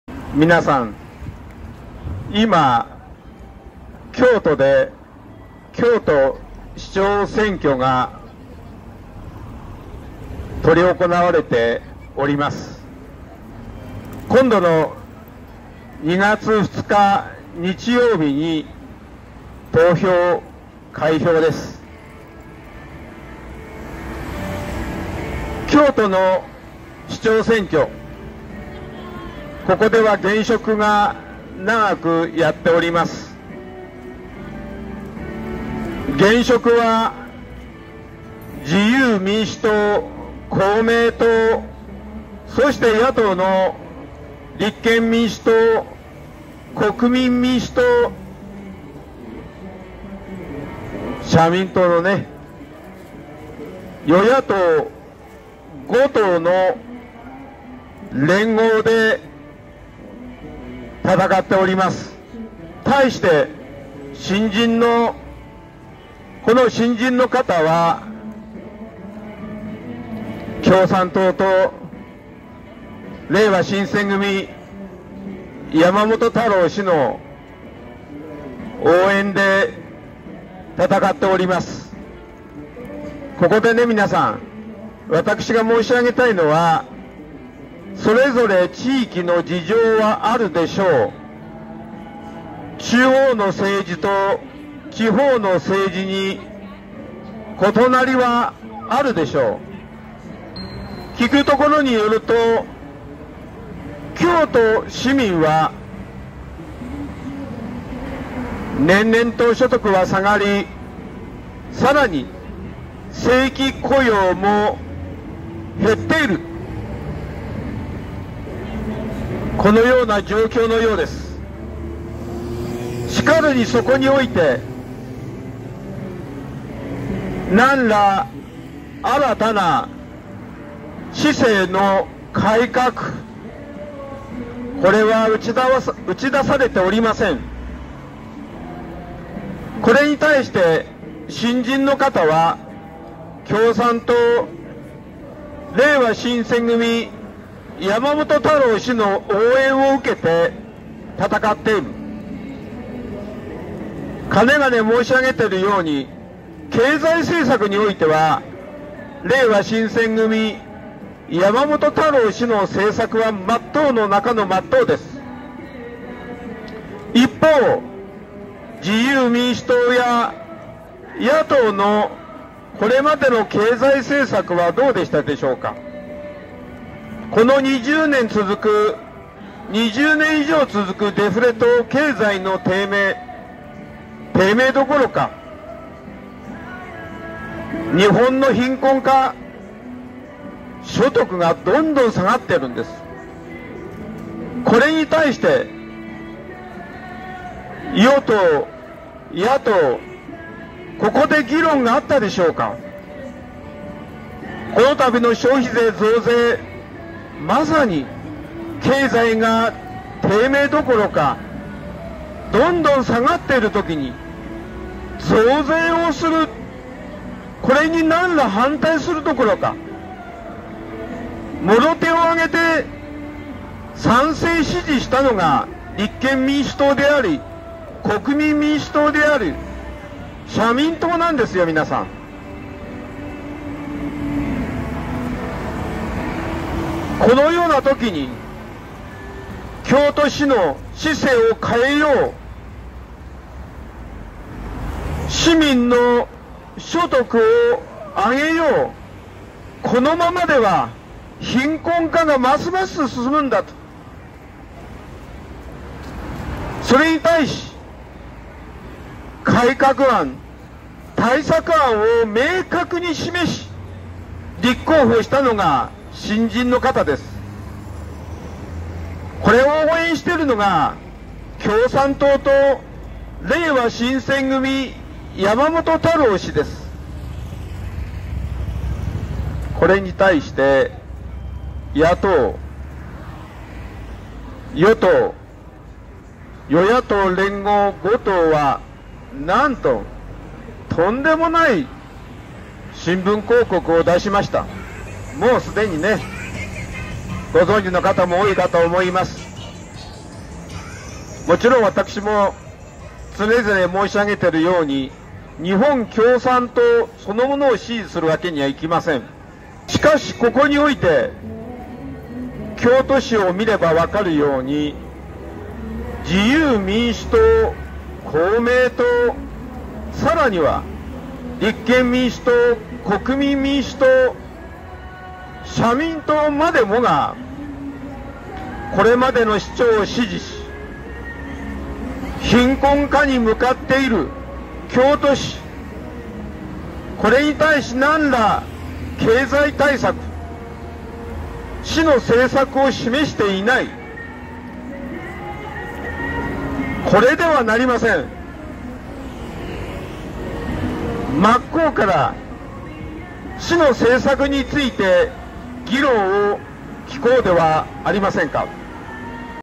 山本太郎氏 貧困脱出 京都市長選新人を推薦応援 現職を自公国立社与野党５党連合推薦20200130木r02新宿駅南口街宣③きみの会はせがわたかし
街頭スピーチ中 新宿駅南口 JR改札前 昭和27年生れ 山形県鶴岡市出身 きみの会 代表世話人はせがわたかし